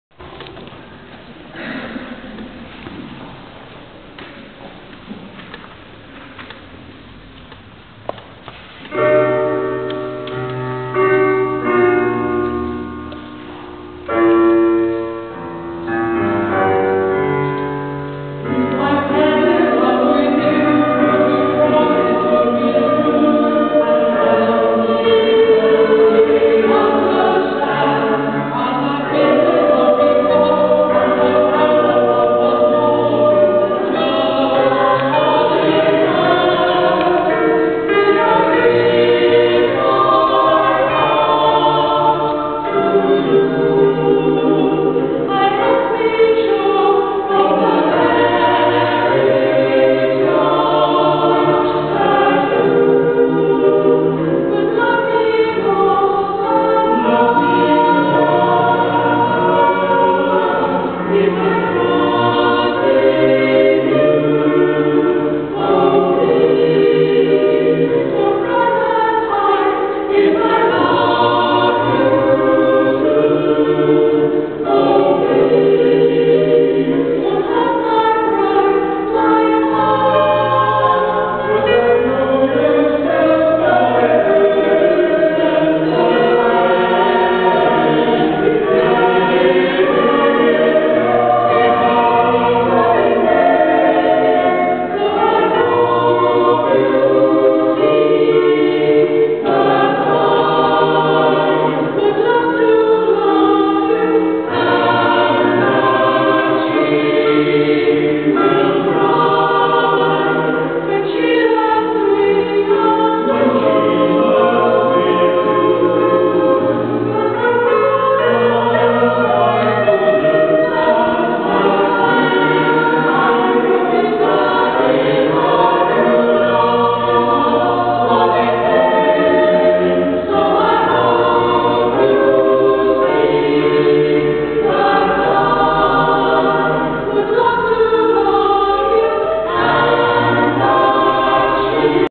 Banovallum Singers IMGP2554
Banovallum Singers Horncastle performing at Bonnetable
Church France  twinning July 2010